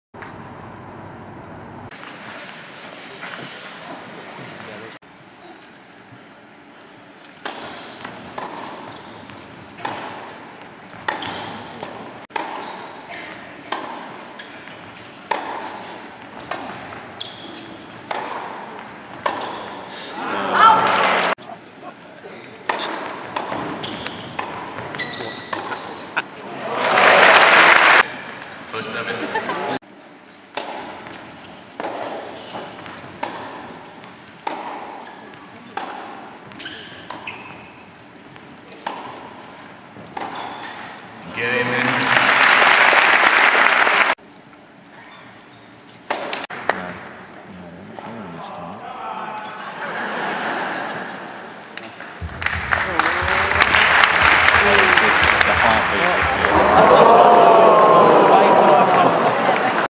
Charity Tennis (Part 1) Prince William, Djokovic, Ivanišević, Henman, Borg,McEnroe,Becker,Wilander
Charity Tennis (Part 1) with Prince William, Djokovic, Goran Ivanišević, Tim Henman, John McEnroe, Mansour Bahrami, Bois Becker, Pat Cash, Bjion Borg, Jonathan Ross, Paul McName, Mats Wilander